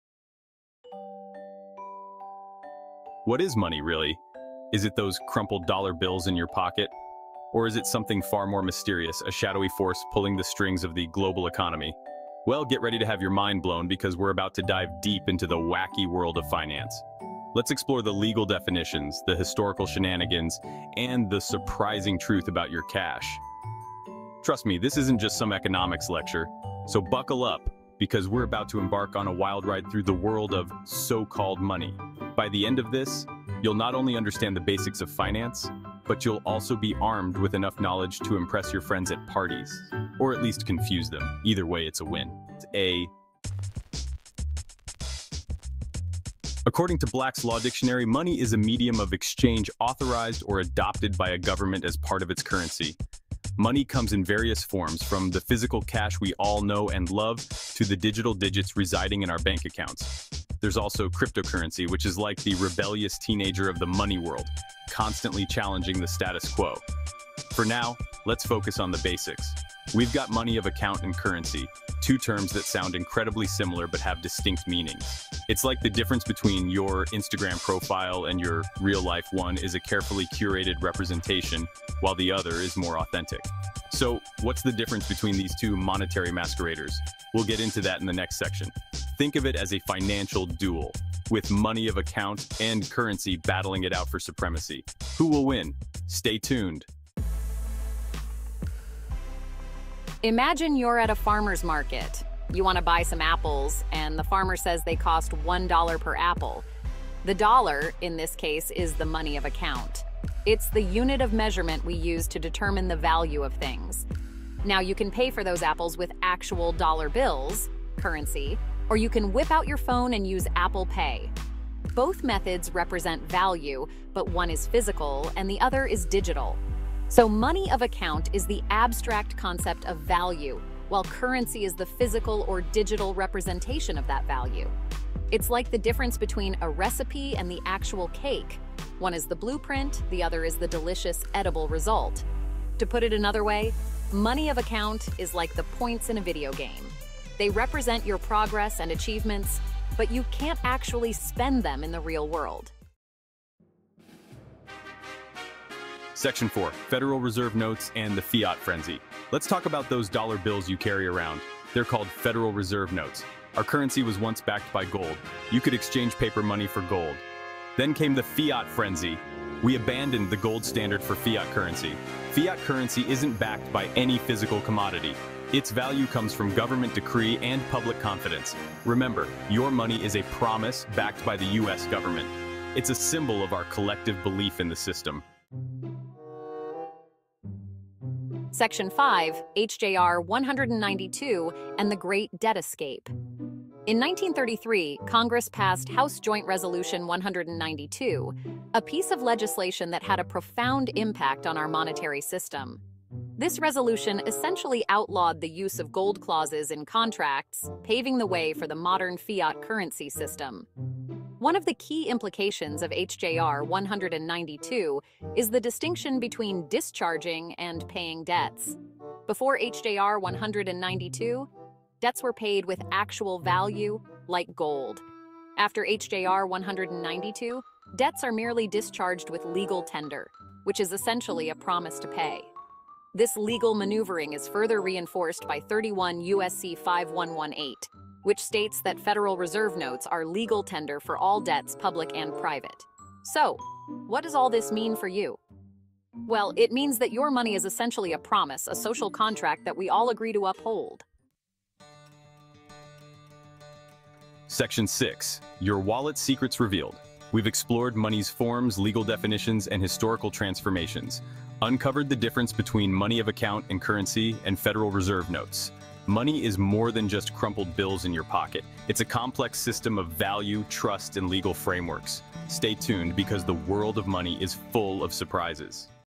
Step 1 : The Basics of Money and Financial Instruments
Money 101: The Truth About Your Cash!

OUTLINE:

00:00:00 What is Money, Really?
00:00:43 More Than Just Dollar Bills
00:01:37 Apples and Oranges (or Maybe Apples and Apple Pay?)
00:02:32 Federal Reserve Notes and the Fiat Frenzy
00:03:05 HJR 192 and the Great Debt Escape
00:04:11 Your Wallet's Secrets Revealed